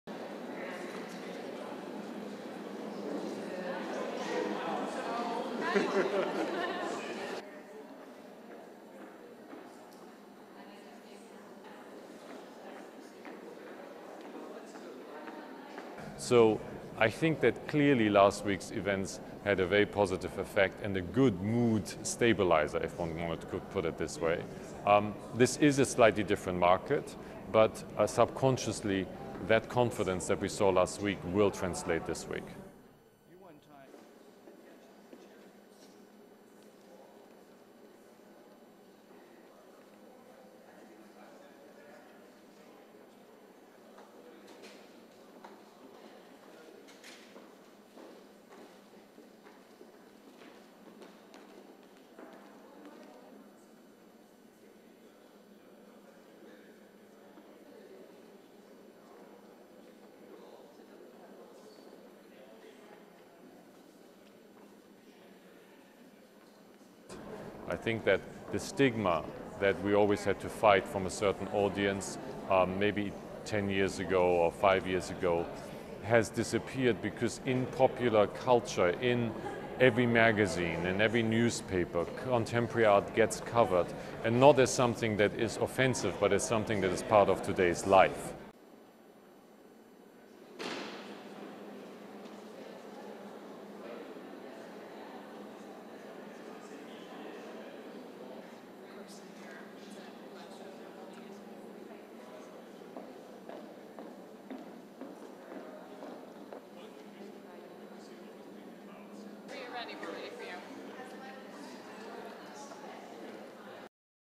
Sotheby's presents spring 2004 contemporary sale in NY
1. Wide pan of gallery starts with Maurizio Cattelan's 'Ballad of Trotsky', ends Ellsworth Kelly's 'Chatham XIII'
2. Ellsworth Kelly's 'Chatham XIII'
3. Close-up Ellsworth Kelly's 'Chatham XIII'
4. SOUNDBITE (English) Tobias Meyer, head of Sotheby's Contemporary Art department:
"I think that clearly last week's events had a very positive effect and a good mood stabiliser if one wanted to put it this way.  This is a slightly different market, but subconsciously that confidence we saw last week will translate this week."
5. Wide gallery
6. Roy Lichtenstein's 'Step-on can with leg'
7. Close-up 'Step-on can with leg'
8. Various Clyfford Still's '1960-F'
9. Woman walking by paintings
10. SOUNDBITE (English) Tobias Meyer, head of Sotheby's Contemporary Art department:
"I think the stigma we always had to fight from a certain audience maybe ten years ago or five years ago has disappeared because in popular culture, in every magazine, in every newspaper, contemporary art gets covered and not as something that is offensive but as something that is part of today's life."
11.Jasper John's 'Corpse and Mirror'
13. Close-up 'Corpse and Mirror'
14.  Donald Judd's 'Untitled' sculpture
15. Maurizio Cattelan's 'The Ballad of Trotsky'
16. Close-up Maurizio Cattelan's 'The Ballad of Trotsky'
17. Wide shot gallery

TOP PRICES EXPECTED AT NY ART SALES

With the art market booming, Sotheby's auction house in New York anticipates heavy bidding when it holds its sale of contemporary art on Wednesday.

At this sale the world famous auctioneer's will try to build on the success of last week which included the record breaking 104 million dollar sale of Picasso's 'Garcon a la Pipe.'

Tobias Meyer, head of Sotheby's Contemporary Art department, believes the momentum is there to have another successful week.

Top to go under the hammer include Ellsworth Kelly's 'Chatham XIII' An L-shaped ellow andred combination that is expected to sell between $1,200,000 and $1,800,000.

Roy Lichtenstein's 'Step-on can with leg' which sold at Sotheby's in November 2002 for $4.9 million will be on the block again and is expected to bring in a similar price between $4,000,000 and $5,000,000.

Clyfford Still's '1960-F' is a physically huge painting taking up nearly an entire wall of the gallery at Sotheby's.  The price is expected to be huge as well, between $3,000,000 and $4,000,000.

Jasper John's watercolour 'Corpse and Mirror' has a low estimate of $2,000,000 and a high estimate of $3,000,000.

Donald Judd's minimalist  'Untitled' sculpture of galvanised iron and blue plexiglas is estimated to sell between $800,000 and $1,200,000.

Finally while not the most important or valuable piece in the sale it is impossible to miss  Maurizio Cattelan's 'The Ballad of Trotsky.'

The full size taxidermied horse and saddle are attached to a pulley system which can hang from the ceiling.

'Trotsky' is expected to be sold for an estimated $600,000-$800,000.